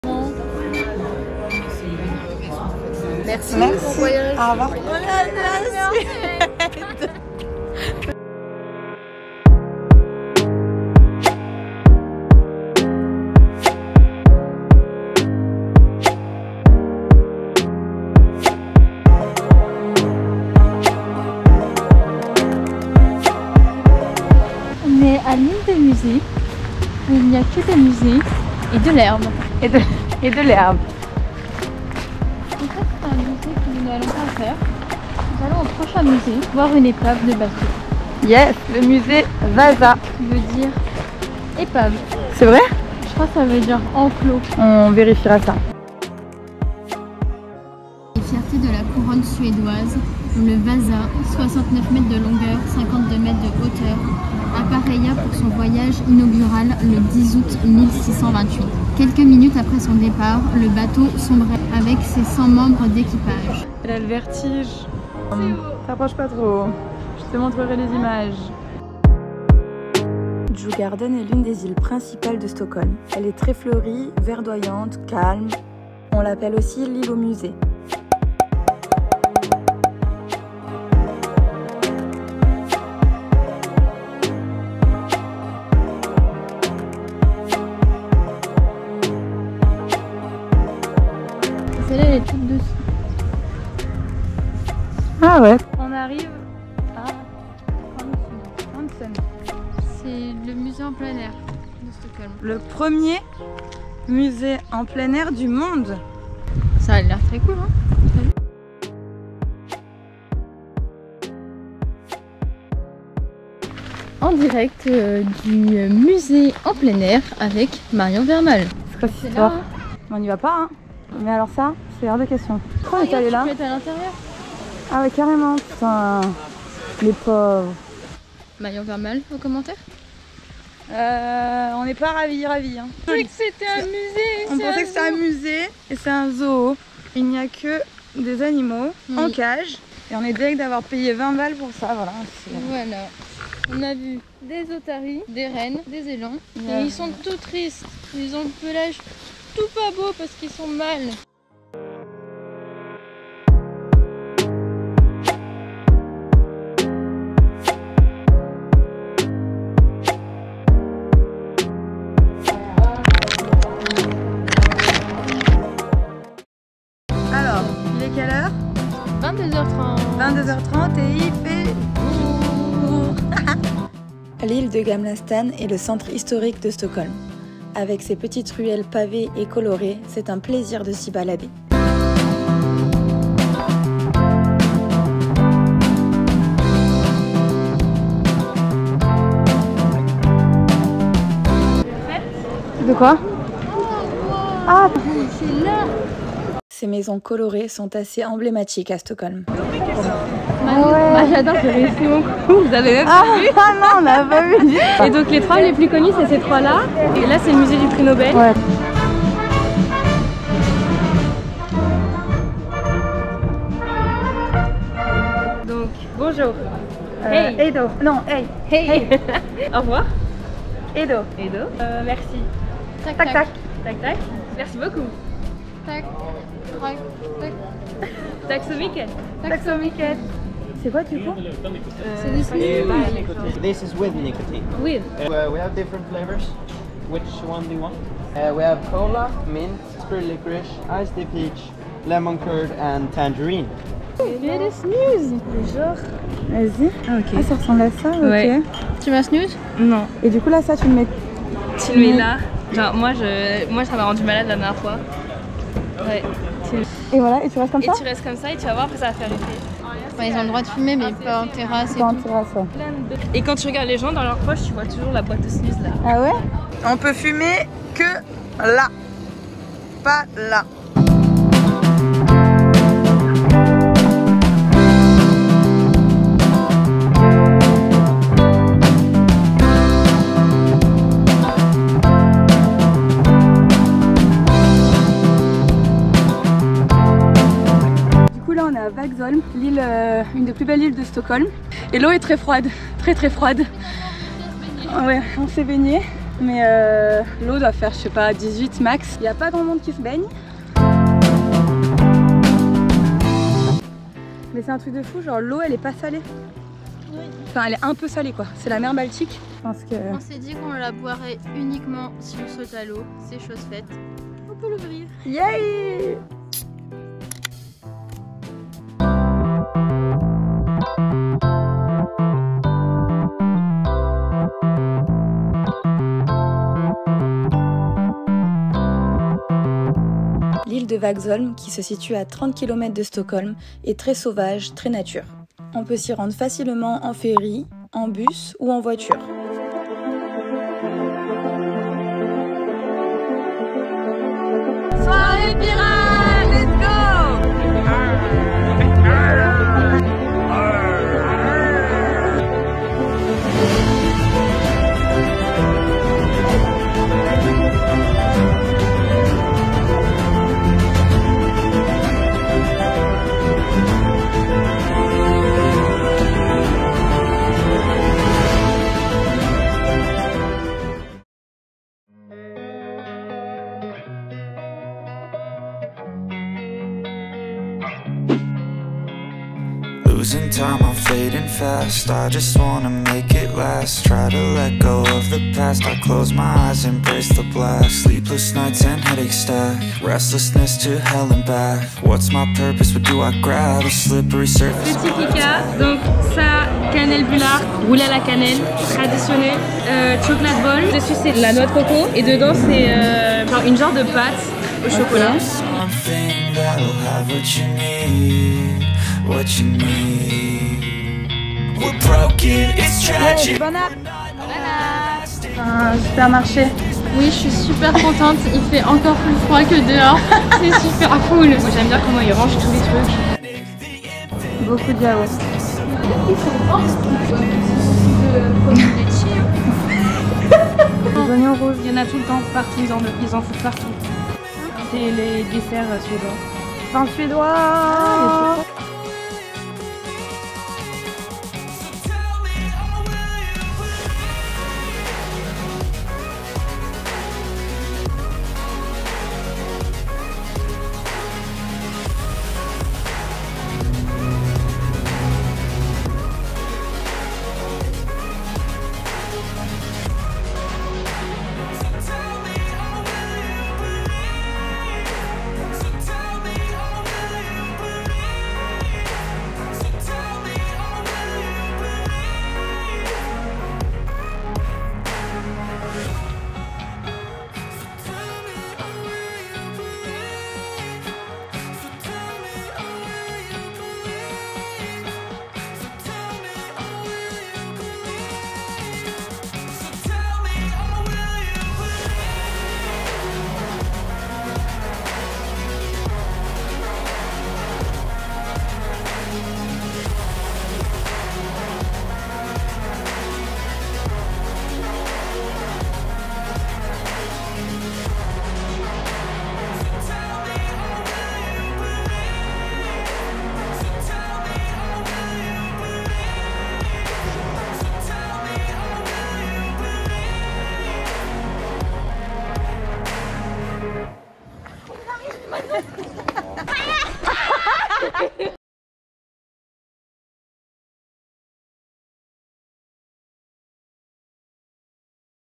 À la découverte de la capitale suédoise : STOCKHOLM (vlog)
Avec Andréa nous sommes parties une semaine rejoindre Manon à Stockholm qui fait un stage de 6 mois. À travers ce vlog, vous allez découvrir la capitale suédoise dans son ensemble :
Quoi faire, quoi voir à Stockholm, la culture suédoise etc…
Voyager dans un pays peu touristique et sous côté à mon goût est tellement plaisant !

Musiques : bibliothèque audio Youtube Studio

Merci !